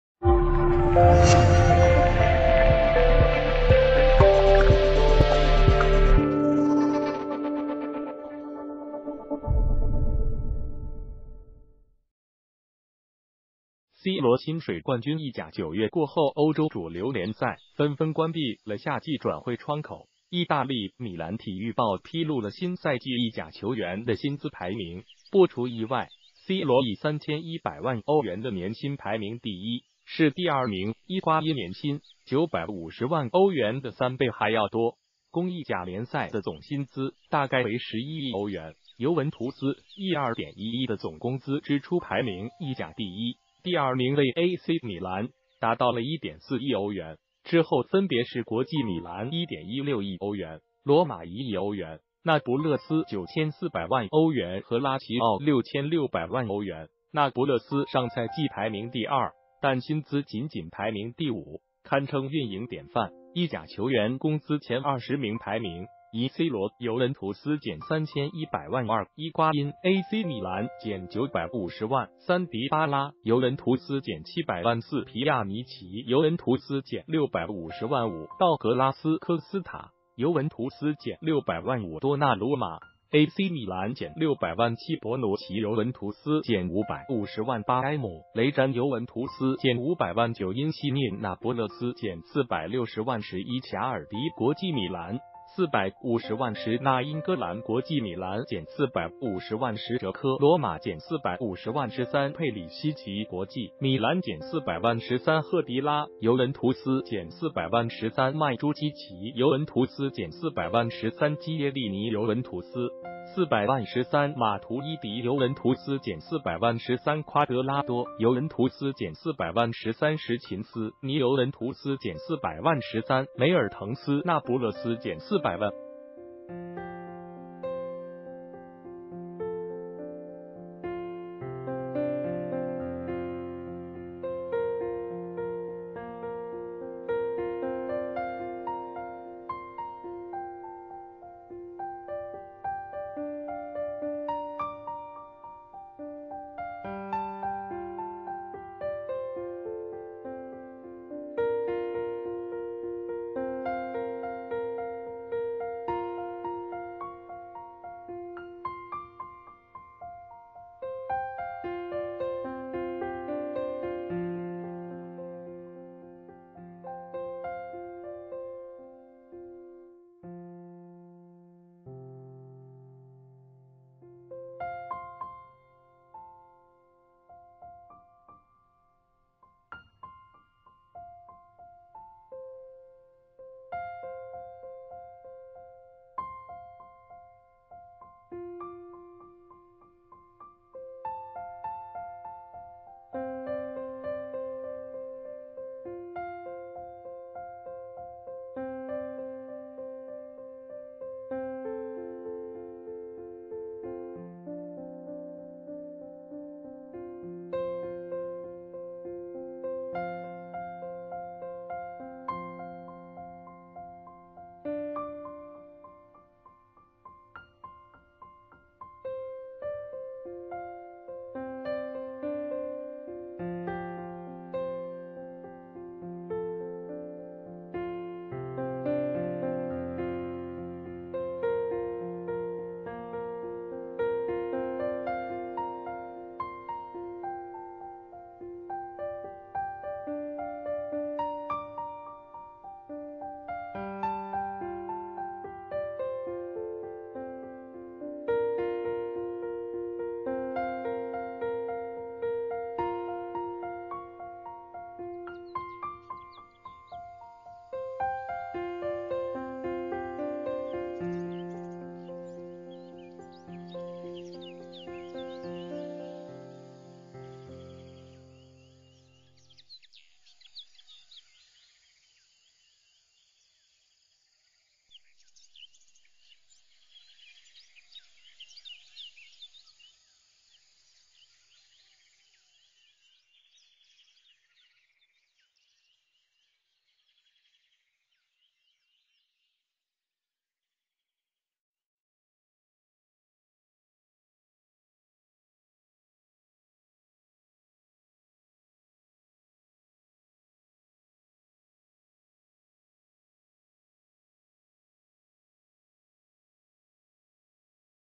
意甲工资榜出炉！C罗冠绝榜首 薪水是第二名3倍
谢谢！C罗薪水冠军意甲9月过后欧洲主流联赛纷纷关闭了夏季转会窗口。意大利《米兰体育报》披露了新赛季意甲球员的薪资排名。不出意外，C罗以3100万欧元的年薪排名第一，是第二名伊瓜因年薪（950万欧元）的3倍还要多。工意甲联赛的总薪资大概为11亿欧元。尤文图斯亿2.1亿的总工资支出排名意甲第一，第二名为AC米兰，达到了1.4亿欧元。之后分别是国际米兰（1.16亿欧元），罗马（1亿欧元），那不勒斯（9400万欧元）和拉齐奥（6600万欧元）。那不勒斯上赛季排名第二，但薪资仅仅排名第五，堪称运营典范。意甲球员工资前20名排名：1C罗（尤文图斯）-3100万2伊瓜因（AC米兰）-950万3迪巴拉（尤文图斯）-700万4皮亚尼奇（尤文图斯）-650万5道格拉斯-科斯塔（尤文图斯）-600万5多纳鲁马（AC米兰）-600万7博努奇（尤文图斯）-550万8埃姆雷-詹（尤文图斯）-500万9因西涅（那不勒斯）-460万10伊卡尔迪（国际米兰）-450万10纳因戈兰（国际米兰）-450万10哲科（罗马）-450万13佩里西奇（国际米兰）-400万13赫迪拉（尤文图斯）-400万13曼朱基奇（尤文图斯）-400万13基耶利尼（尤文图斯）-400万13马图伊迪（尤文图斯）-400万13夸德拉多（尤文图斯）-400万13什琴斯尼（尤文图斯）-400万13梅尔滕斯（那不勒斯）-400万